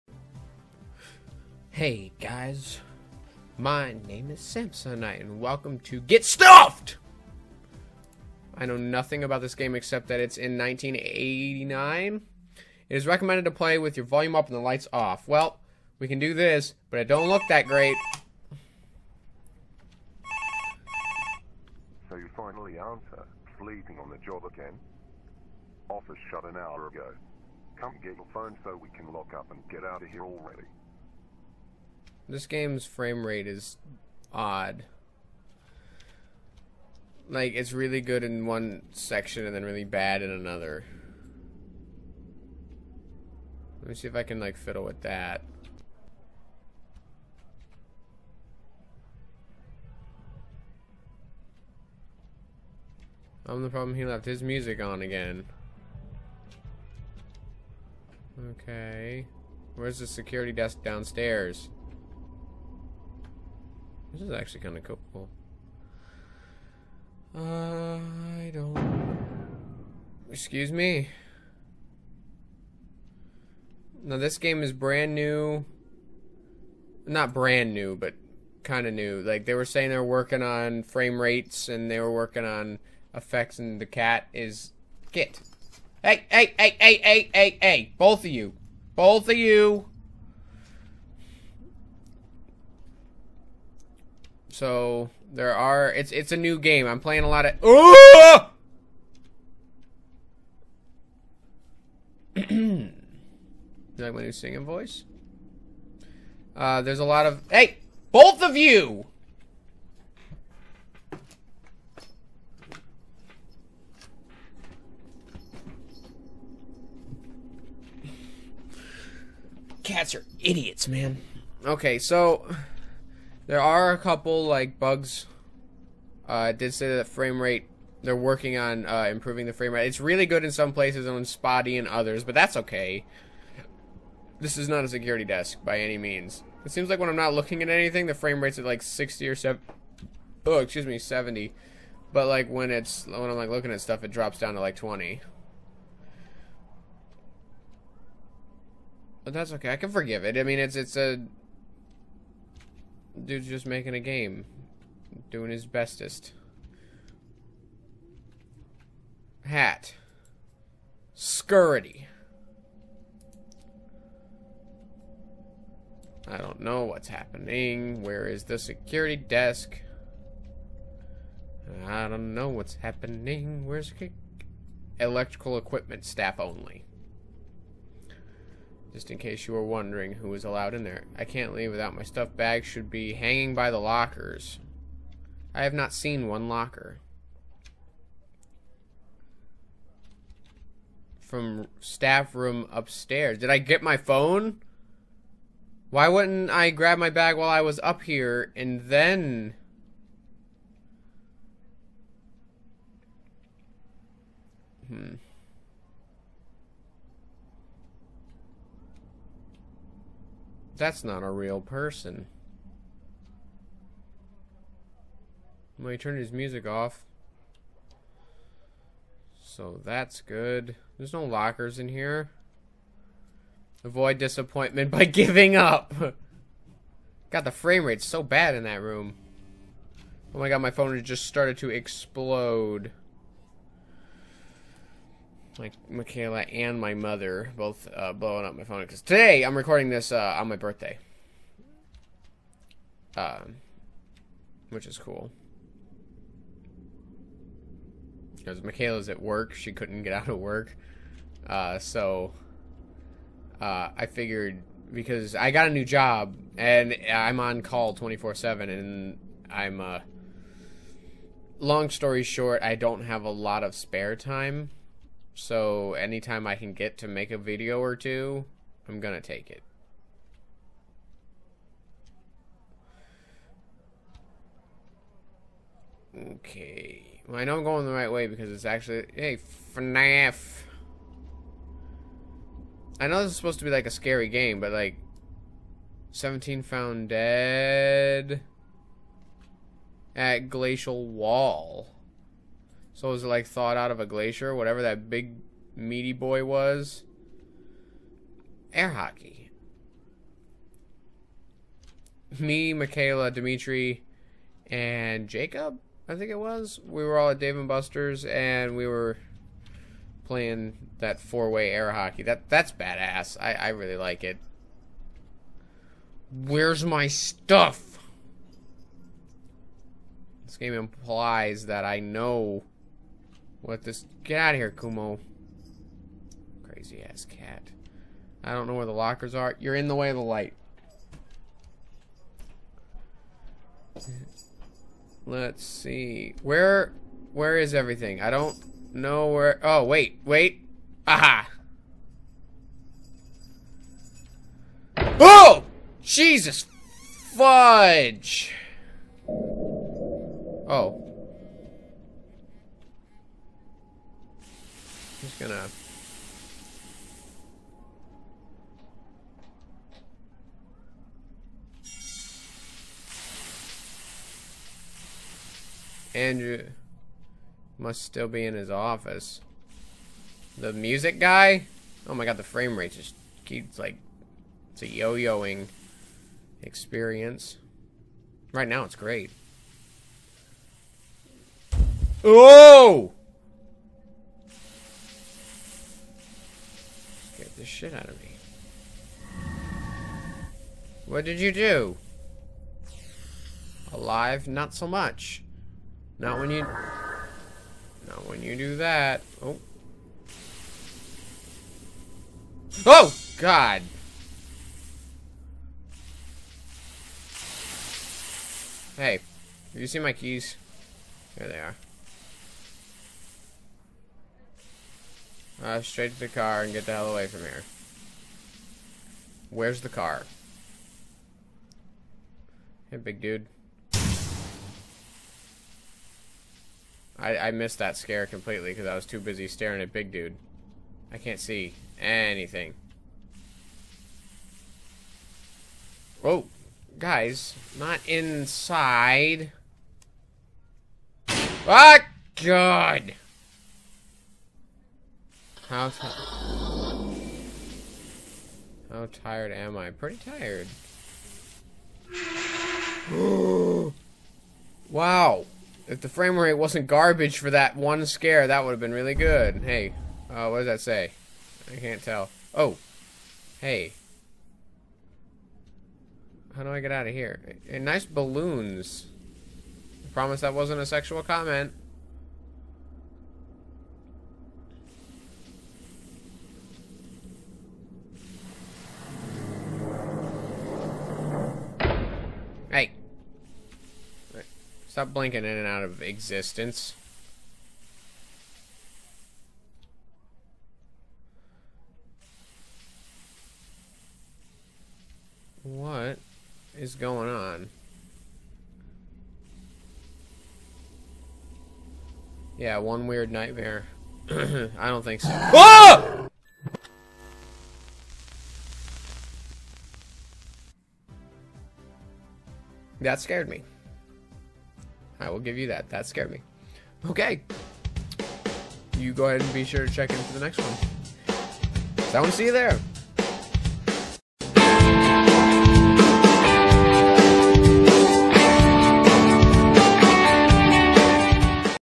LIKE A TURKEY - Get Stuffed
It's Thanksgiving day! Just that...I'm the main course.

We made a movie, and we worked super hard on it, here it is! (Absolute must see)